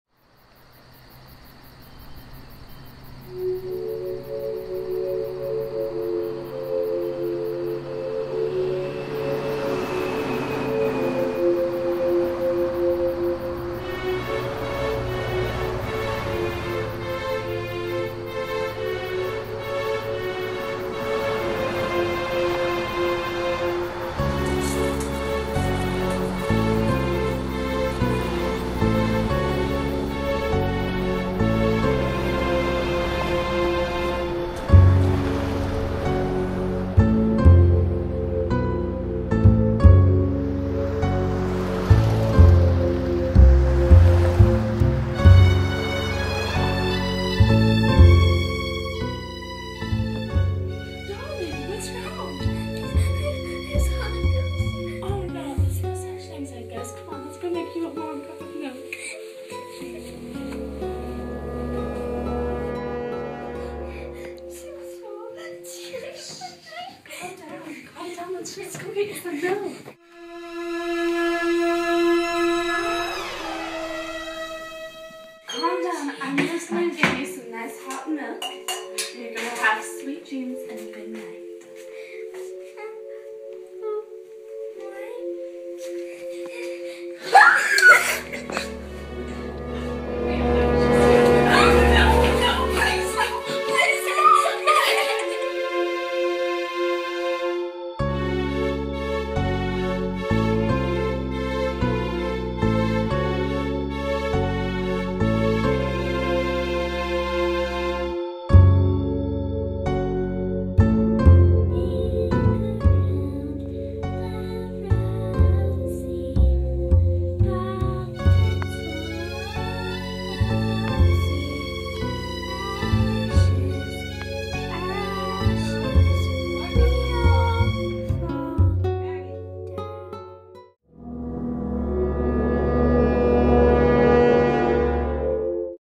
The Convent
My siblings and I decided to make a scary movie with my iPhone while staying an old Nuns Convent in Southern Missouri.